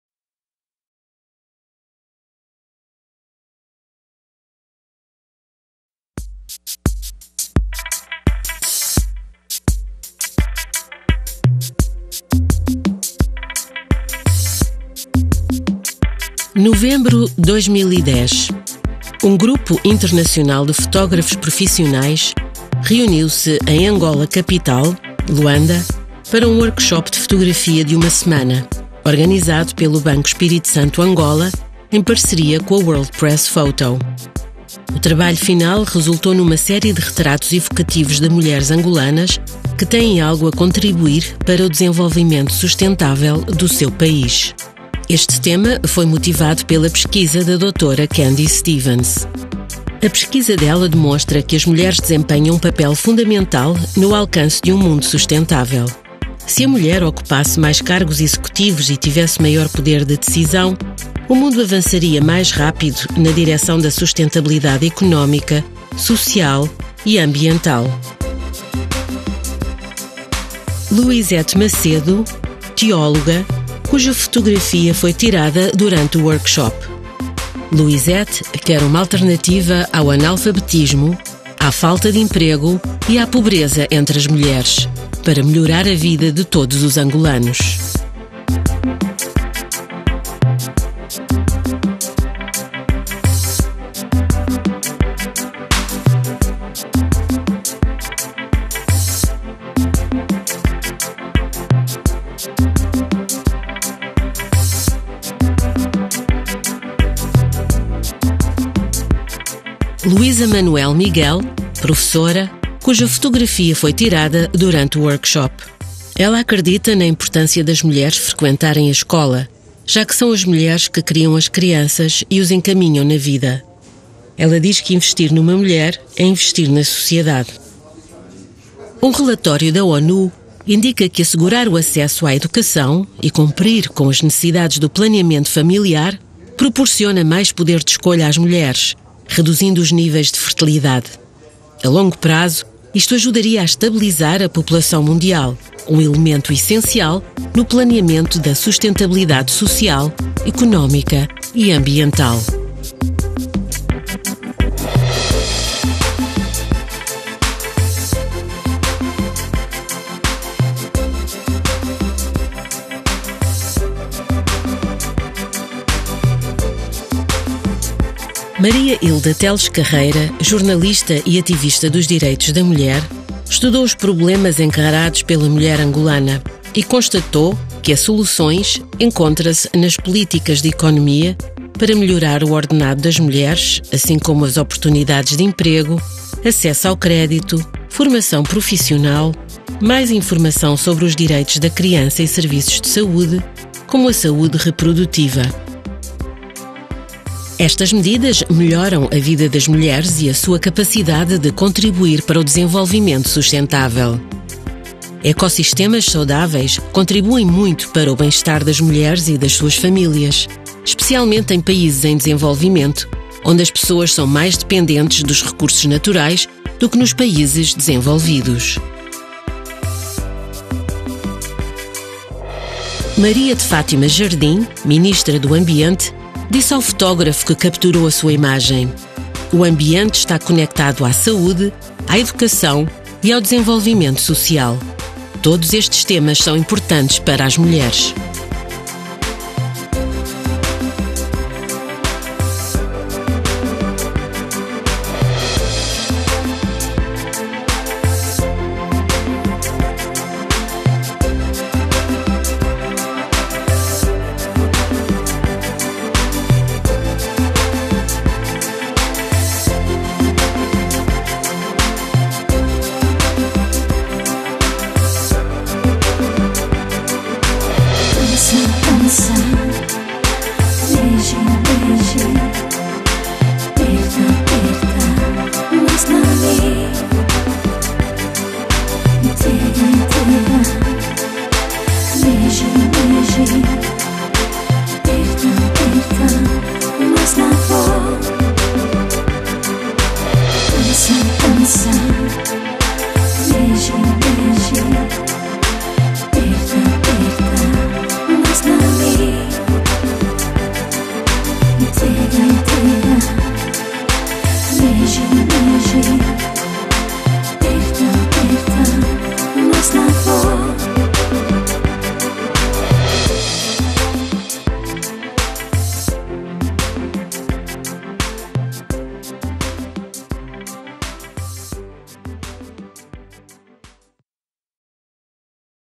Angolan Women: an approach to sustainable development?
Gender equality is a basic human right and the empowerment of women will undoubtedly bring many positive changes (Alvaro Sobrinho, CEO Banco Espirito Santo Angola).

World Press Photo Exhibition 2011, Luanda, Angola